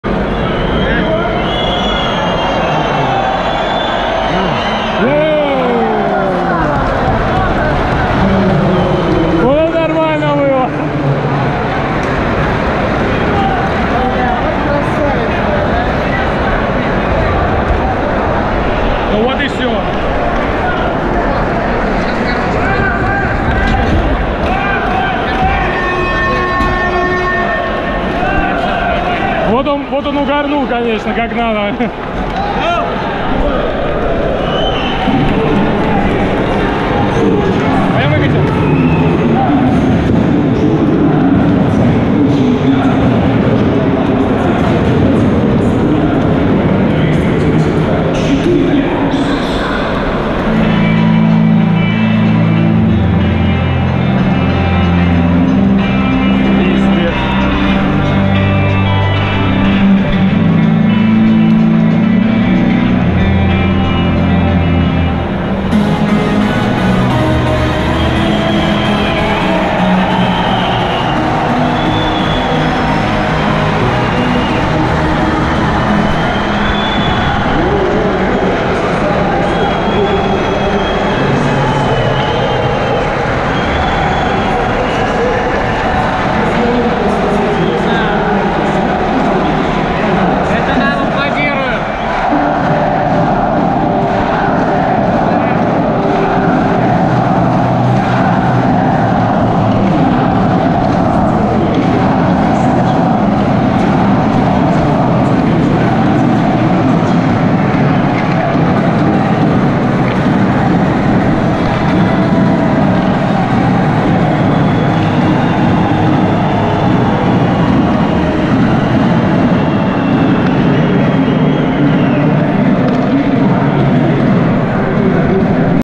Буллит Линуса Умарка
Буллит Линуса Умарка на кубке Губернатора Нижегородской области по хоккею, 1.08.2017